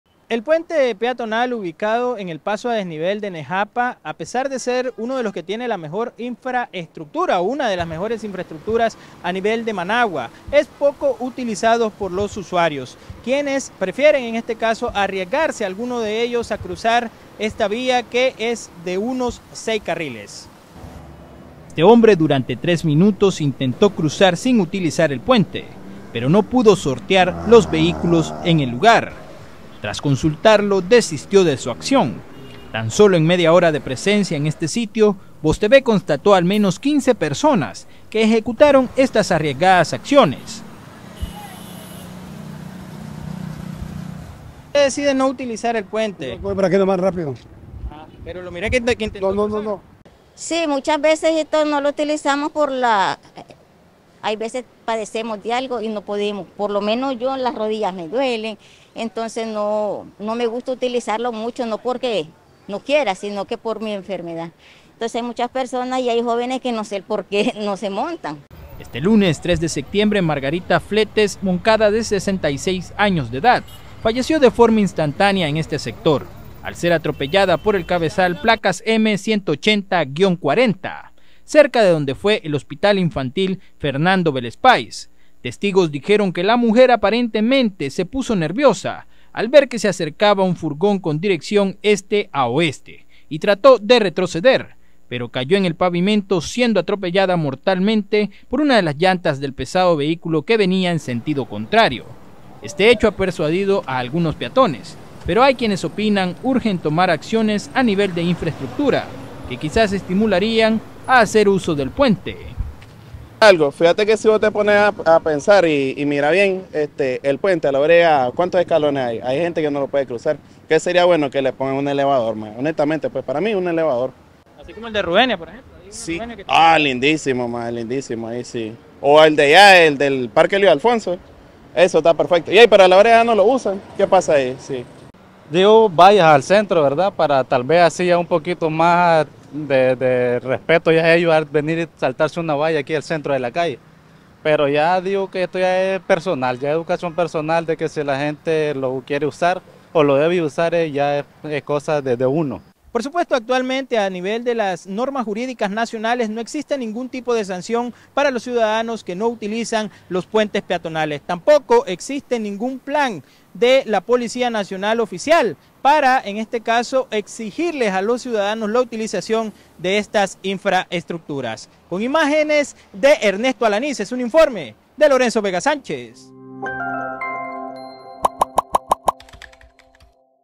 El poco uso del puente peatonal en Nejapa sigue dejando tragedias
A pesar de que el puente peatonal, ubicado en el paso a desnivel de Nejapa, en Managua, tiene una buena estructura, es poco utilizado por la población, lo que implica un riesgo de que las personas se vean involucradas en un accidente de tránsito al cruzar los seis carriles vehiculares en la zona.